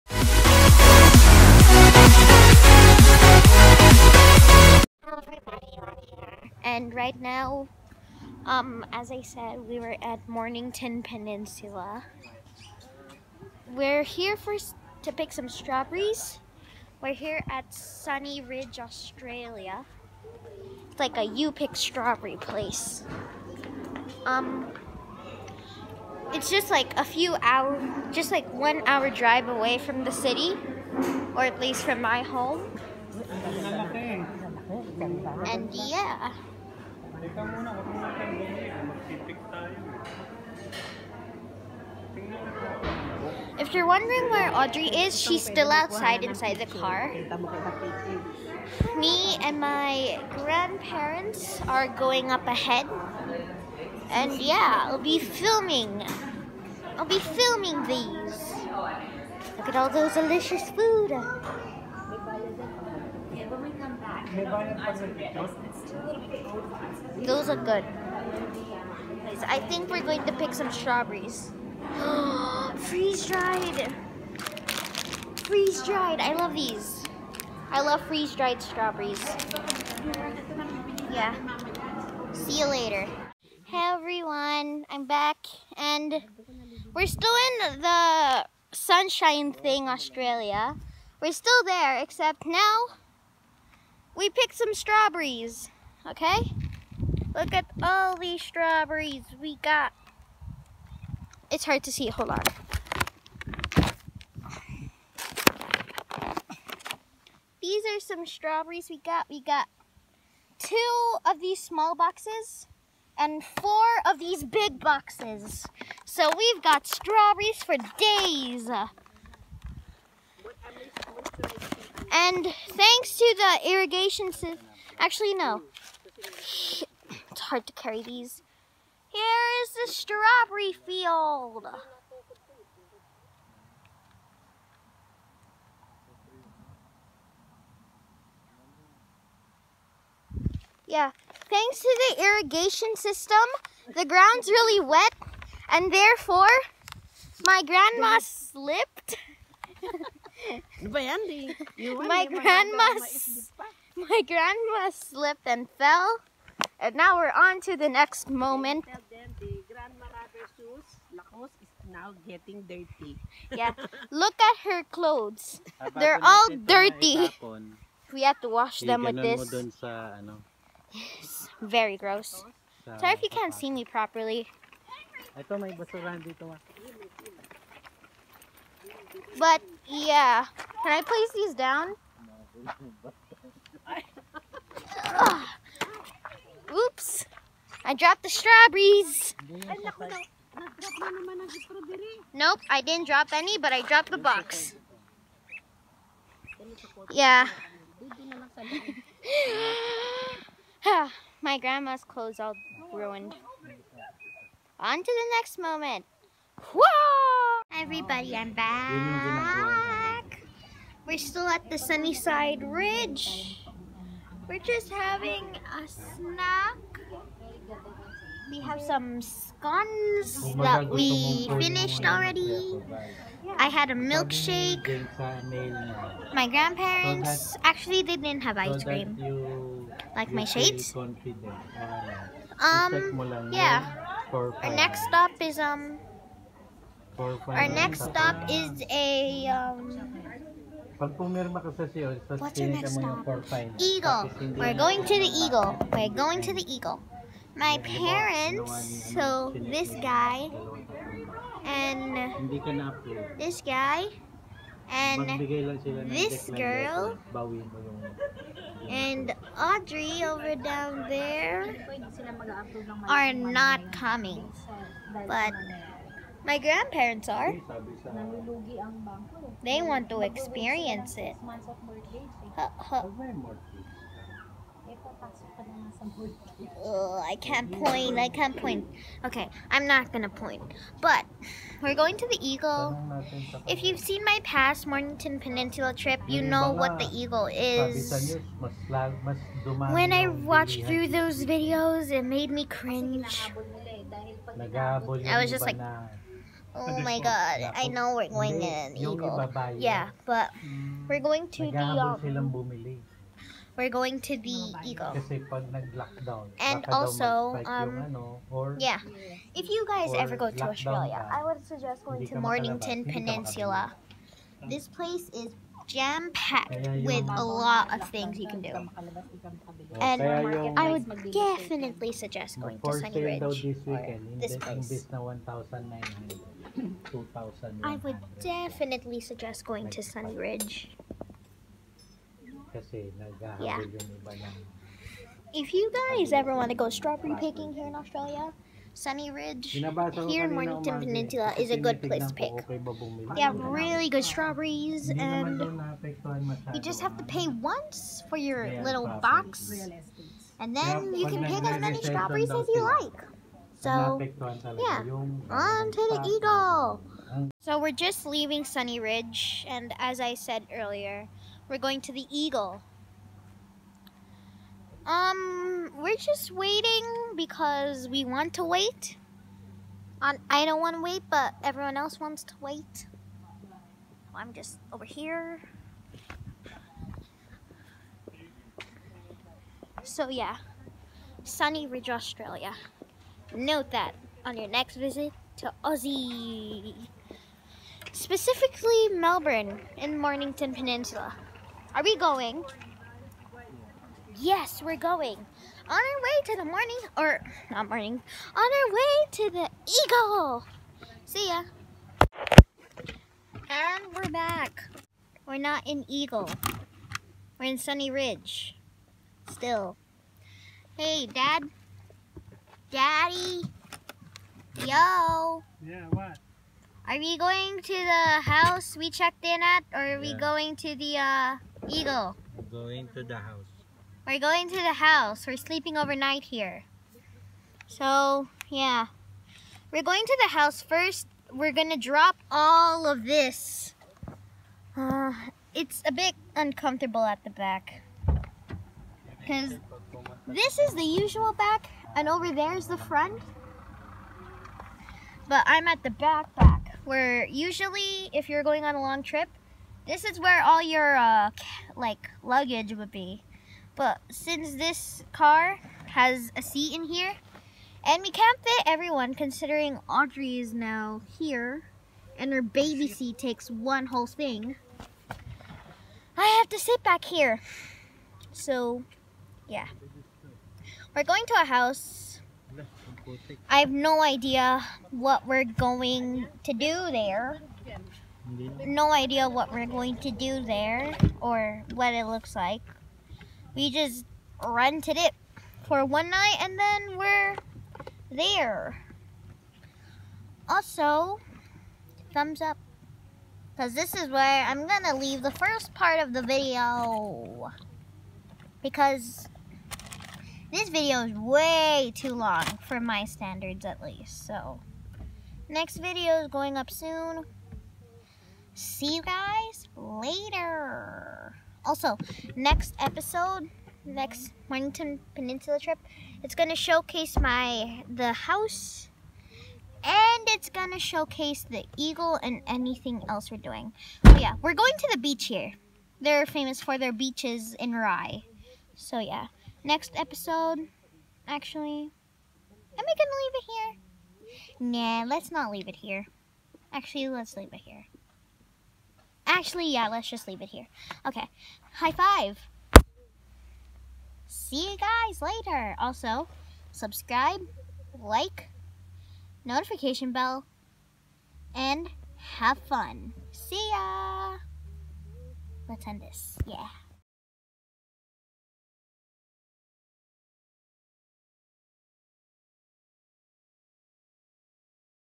Mornington Peninsula (Sunny Ridge) Ep.1
I go on my adventure back to the peninsula with my Grandparents. First destination, Sunny Ridge. I highly recommend this place